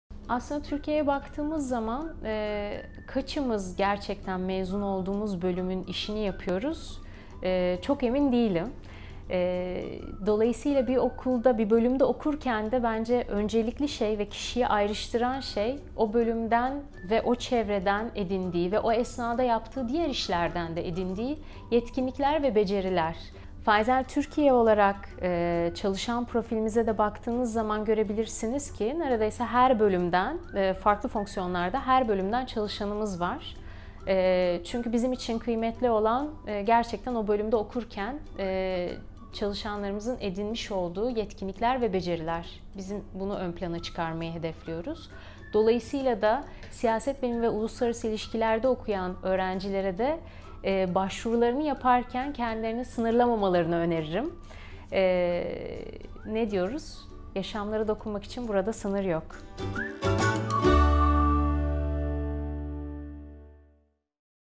Siyaset Bilimi ve Uluslararası İlişkiler bölümü mezunu ne iş yapar? İş imkanları nelerdir?
Pfizer - İnsan Kaynakları Müdürü Seda Tamur Oğralı Anlatıyor!